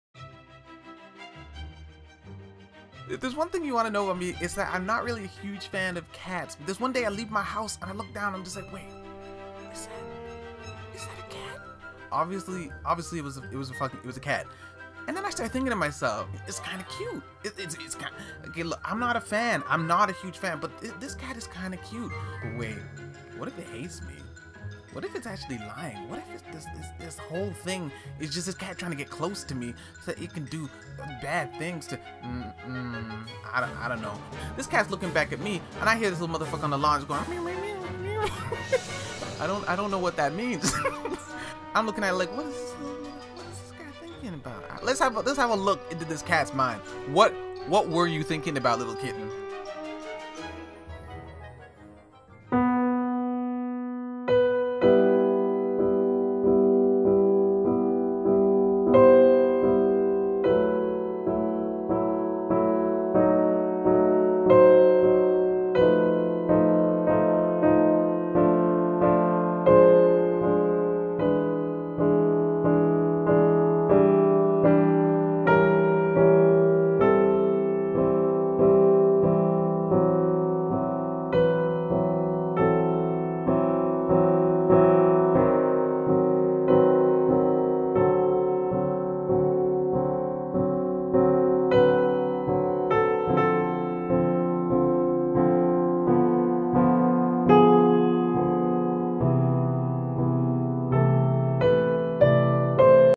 KITTY BETRAYAL! | Animatic Short Story
While working on my comedy, I made this short story. I'm still not very good at comedy yet, but I think this came out well! I also realized my humor gets so dark! And no, not because i'm black!